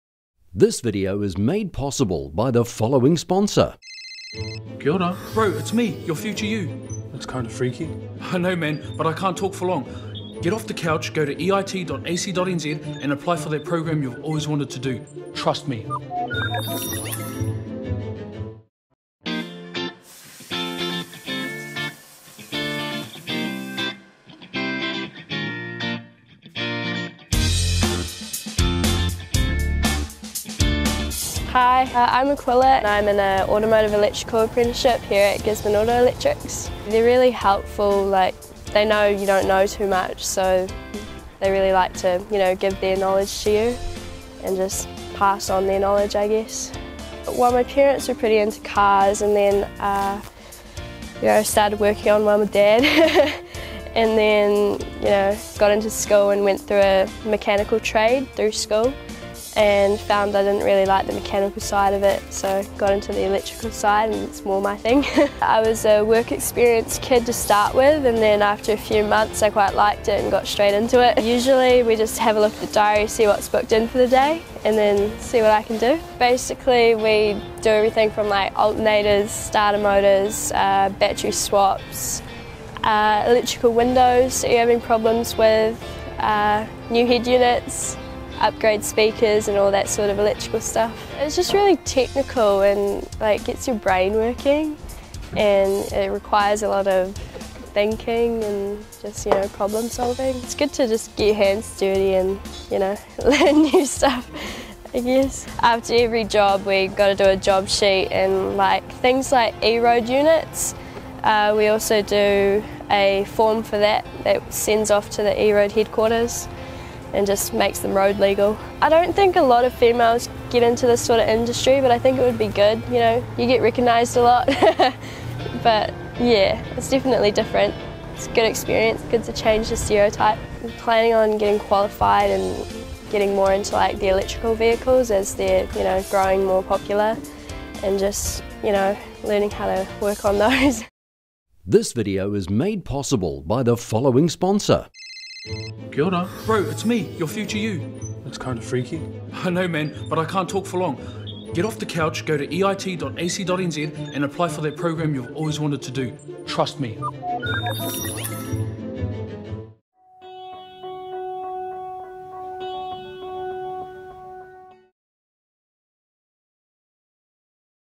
Aquila's Journey as a Female Apprentice in the Mechanics Industry
Aquila breaks gender barriers as she pursues her apprenticeship as a mechanic in a male-dominated industry. After leaving school, Aquila found her passion in the mechanical and electrical industries. Despite facing gender stereotypes, she continues to strive toward achieving her goals.

Aquila enjoys the technical aspect of it, the challenge of thinking and problem-solving. Aquila also shared that the men in the industry are helpful and are willing to pass on their knowledge to her.

Her parents' love for cars also influenced Aqula's passion for the field. She learned from her dad and even went through a mechanical trade through school. However, she discovered that her interest lies more in electrical work.